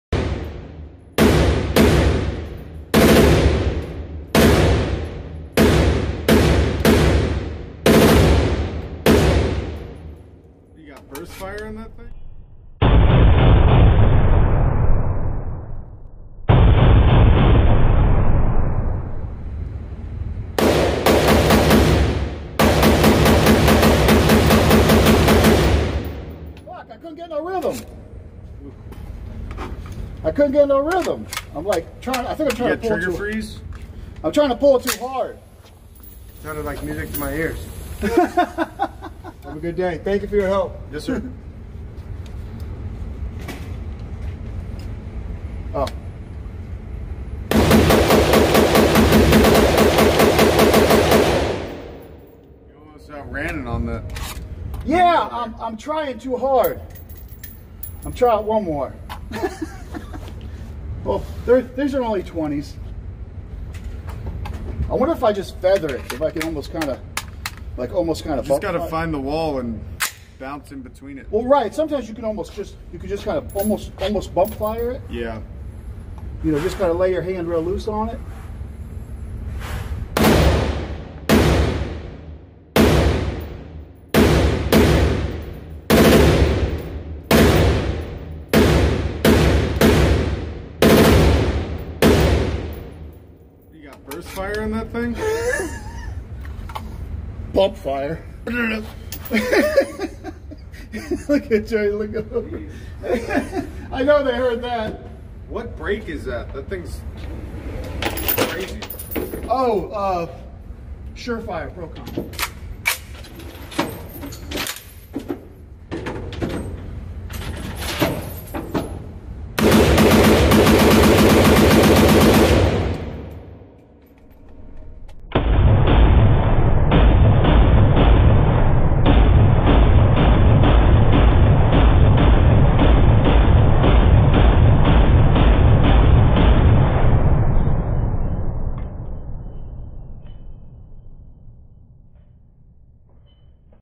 Sig Sauer M400 10.5 - Hiperfire Hipertouch competition trigger, burst fire. Surefire Procomp muzzle
Sig Sauer M400 10.5 - Hiperfire Hipertouch competition trigger, Surefire Procomp muzzle brake.
Maxim Defense PDX 7.62x39.
Bumpfire, rapid fire, mag dumps.

Sunday Gun Day
Ammo Shortage 2020